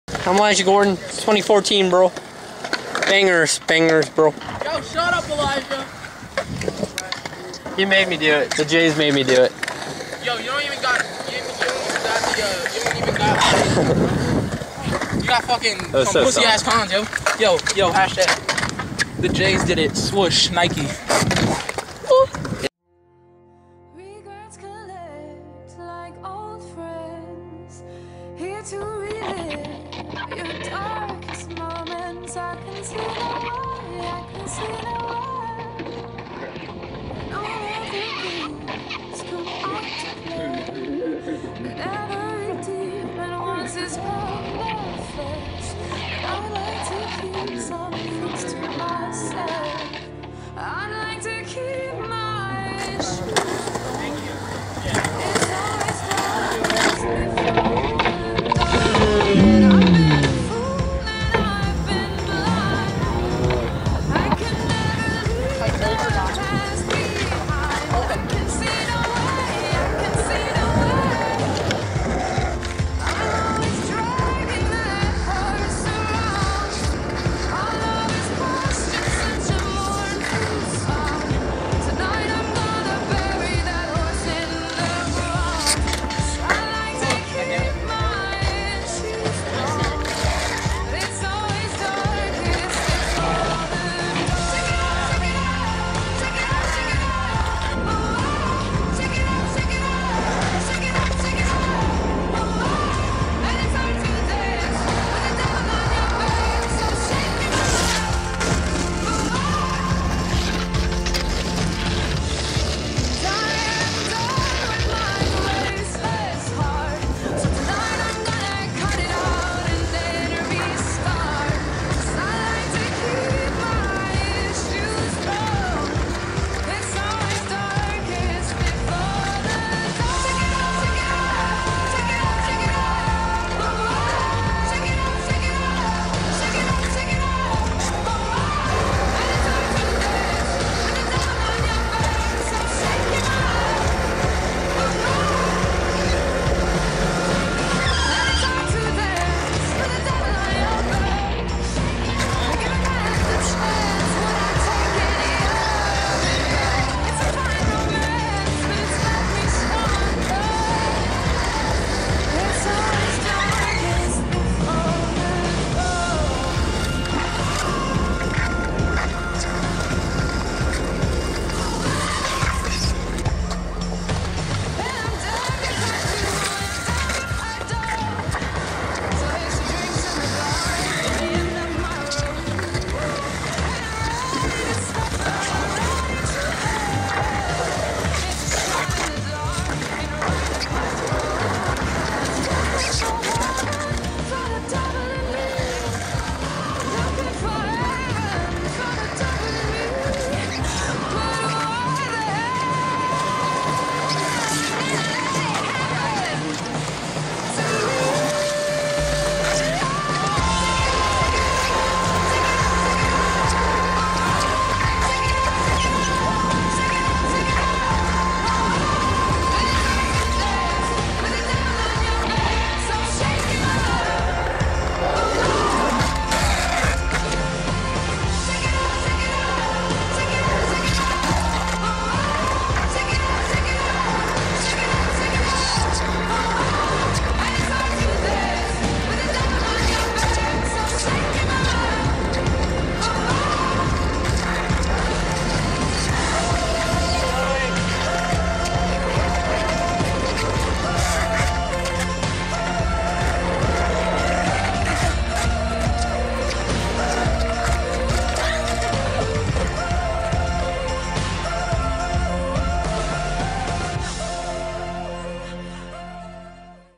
Laurel with the Boizzz 3
Newest Laurel with the boizz, this was really fun to film and edit. Featuring Alex Bass, Elijah Gordon, Michael McGraw, Jake Kushner, Taylor Barrett, Nate Allen, Nnamdi ihekwoaba, and Wes Chambers.